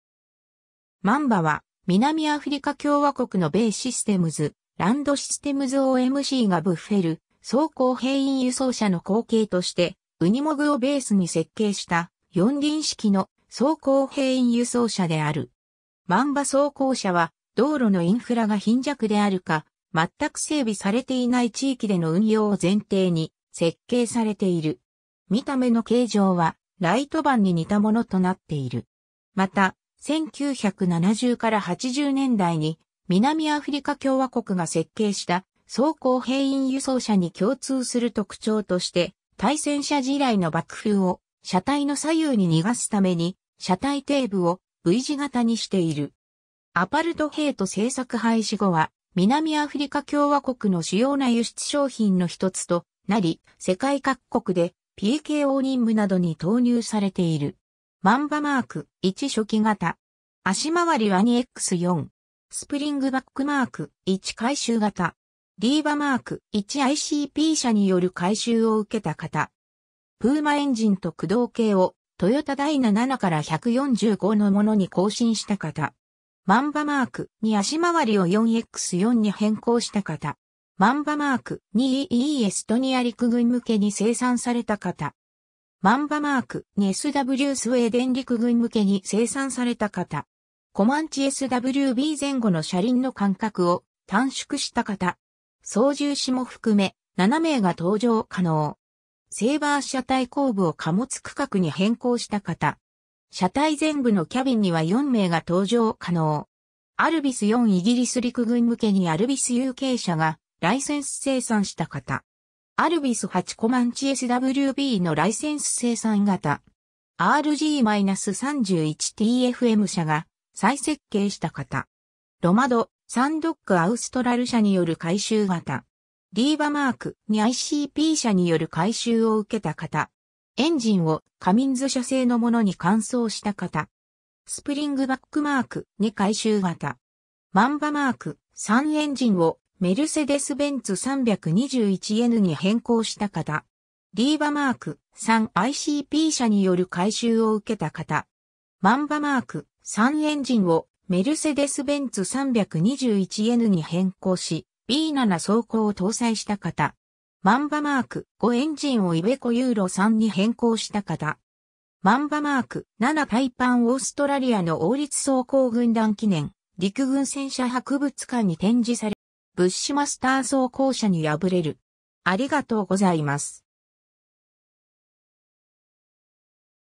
マンバ装甲兵員輸送車
マンバ（Mamba）は、南アフリカ共和国のBAEシステムズ・ランドシステムズ OMCがブッフェル装甲兵員輸送車の後継としてウニモグをベースに設計した、四輪式の装甲兵員輸送車である。
 マンバ装甲車は、道路のインフラが貧弱であるか、全く整備されていない地域での運用を前提に設計されている。
見た目の形状はライトバンに似たものとなっている。
 また、1970-80年代に南アフリカ共和国が設計した装甲兵員輸送車に共通する特徴として、対戦車地雷の爆風を車体の左右に逃がすために車体底部をV字型にしている。
 アパルトヘイト政策廃止後は南アフリカ共和国の主要な輸出商品の1つとなり、世界各国でPKO任務などに投入されている。
 マンバ Mk.1 初期型。
足回りは2x4。
 スプリングバック Mk.1 改修型。
 リーバ Mk.1 ICP社による改修を受けた型。
 プーマ エンジンと駆動系をトヨタ・ダイナ 7-145の物に更新した型。
 マンバ Mk.2 足回りを4x4に変更した型。
 マンバ Mk.2 EE エストニア陸軍向けに生産された型。
 マンバ Mk.2 SW スウェーデン陸軍向けに生産された型。
 コマンチ SWB 前後の車輪の間隔を短縮した型。
操縦士も含め、7名が搭乗可能。
 セイバー 車体後部を貨物区画に変更した型。
車体前部のキャビンには4名が搭乗可能。
 アルビス4 イギリス陸軍向けにアルビスUK社がライセンス生産した型。
 アルビス8 コマンチ SWBのライセンス生産型。
 RG-31 TFM社が再設計した型。
 ロマド Sandock Austral社による改修型。
 リーバ Mk.2 ICP社による改修を受けた型。
エンジンをカミンズ社製のものに換装した型。
 スプリングバック Mk.2 改修型。
 マンバ Mk.3 エンジンをメルセデス・ベンツ 321Nに変更した型。
 リーバ Mk.3 ICP社による改修を受けた型。
 マンバ Mk.3 エンジンをメルセデス・ベンツ 321Nに変更し、B7装甲を搭載した型。
 マンバ Mk.5 エンジンをイヴェコ ユーロ3に変更した型。
 マンバ Mk.7 タイパン オーストラリアの王立装甲軍団記念・陸軍戦車博物館に展示されているタイパン装甲車 オーストラリアの特殊車両システム計画（Australian Specialised Vehicle System）の候補として、エンジンをメルセデス・ベンツ OM366LA ターボディーゼルに換装した型。
ブッシュマスター装甲車に敗れる。